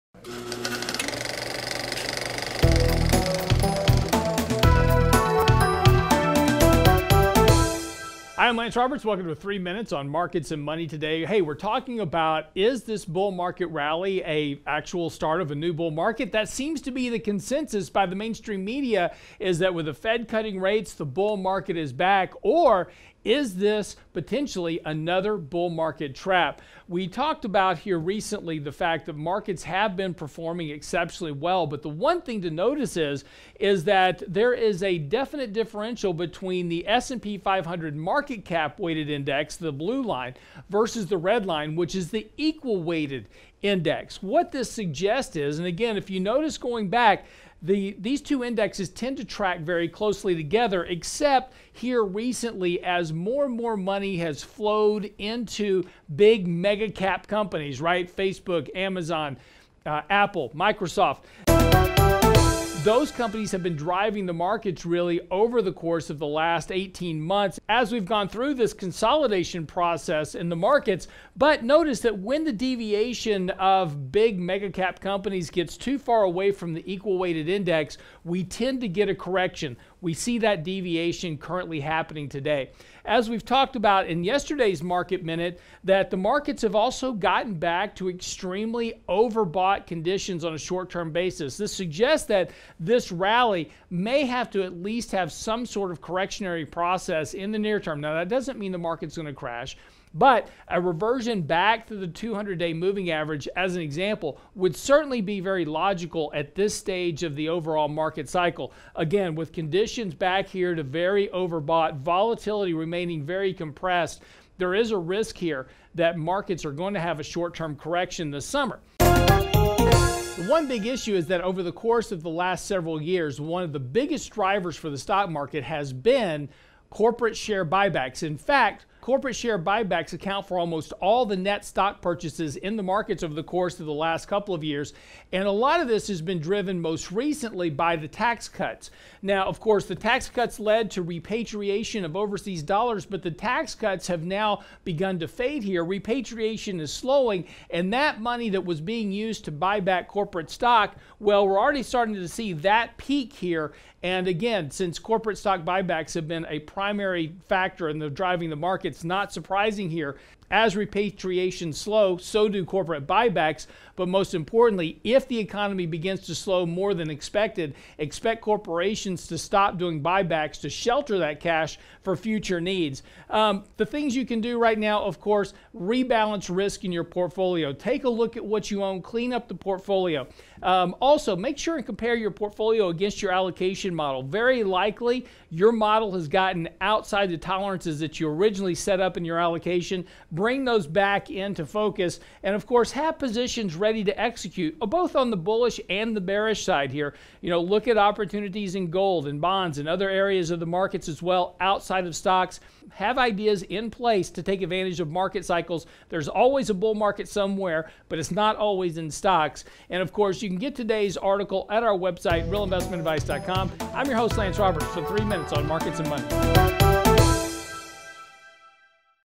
Bull Market or Bear Trap?
RIA Advisors Chief Investment Strategist Lance Roberts examines market performance from a technical perspective to answer this question--Green Light or Red Light?